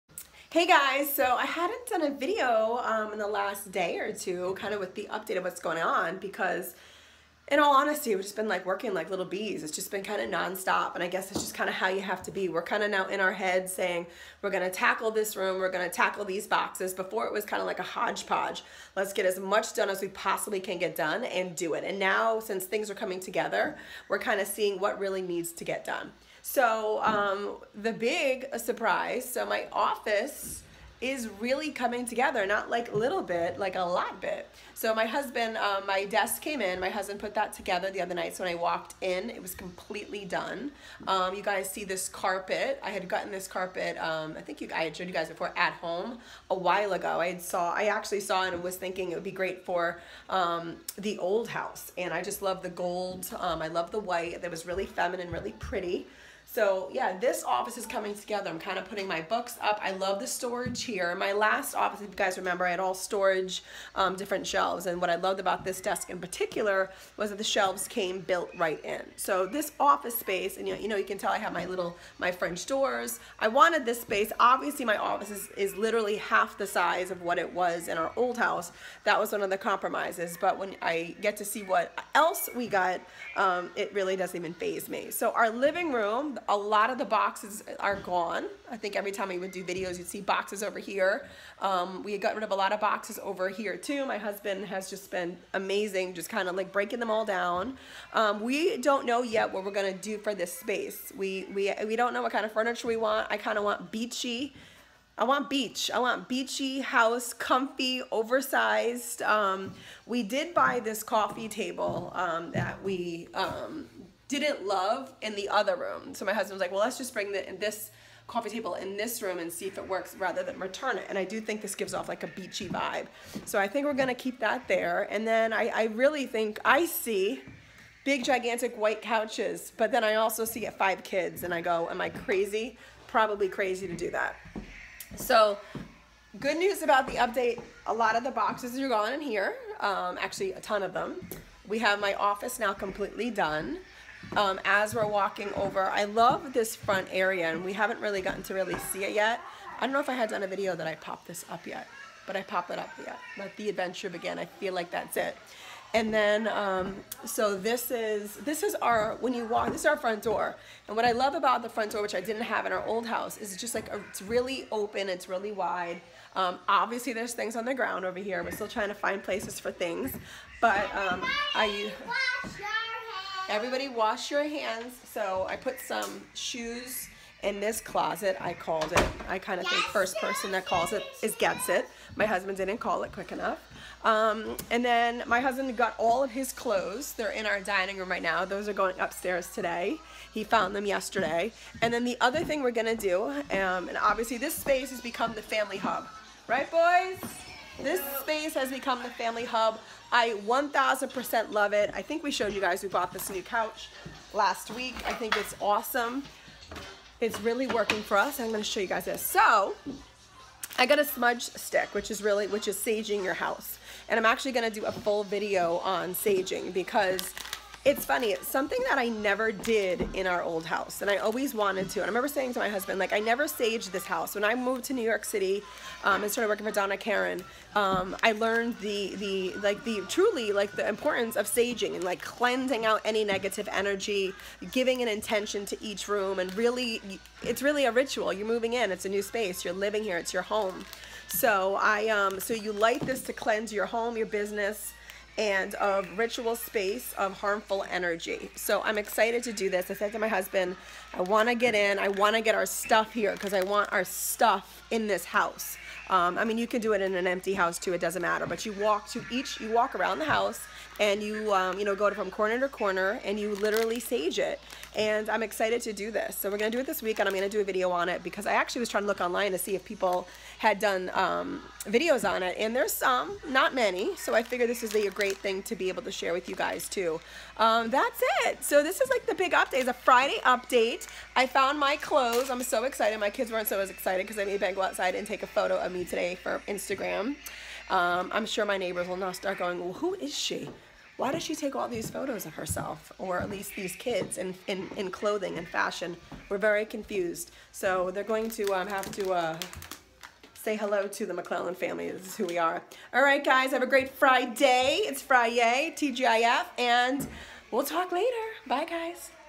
House Update: SURPRISE OFFICE AND ROOMS SET UP!!!
House Update: WE HAVE ROOMS SET UP!!!
I am loving my new office! It's getting there!
My husband surprised me with putting it together!